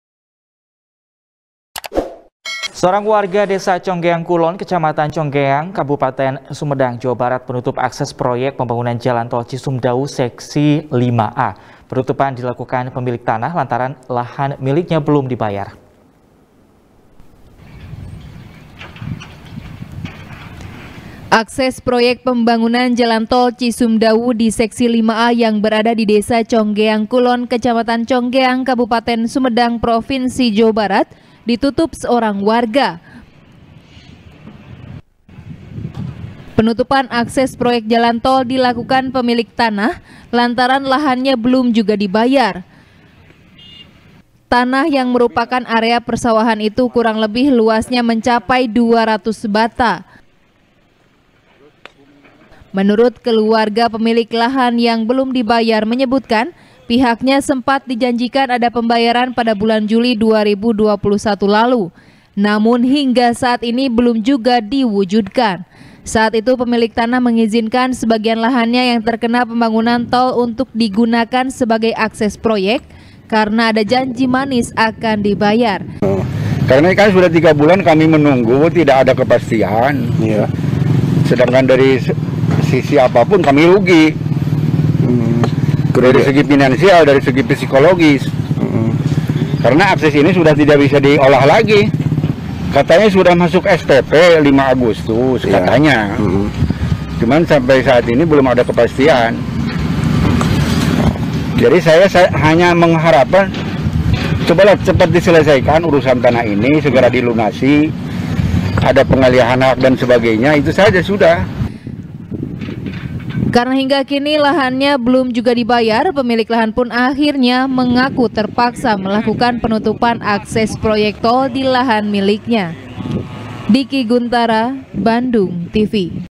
Uang Sewa Belum Dibayar Warga Tutup Akses Pekerja Tol | SBR | BANDUNGTV
SBR | BANDUNGTV | IRFAN RAMANDA | Seorang Warga Desa Conggeang Kulon,Kecamatan Conggeang,Kabupaten Sumedang,Jawa Barat,Menutup Akses Proyek Pembangunan Jalan Tol Cisumdawu Seksi Lima A.Penutupan Dilakukan Pemilik Tanah,Lantaran Lahan Miliknya Belum Dibayar.